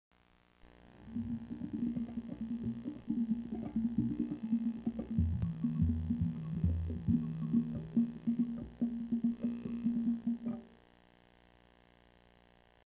MTV "Dancing Paint" Bumper (1995)
Taken during MTV's "My So-Called Life Marathon" from late April/Early May 1995.
Side note, I don't really know what this one's called.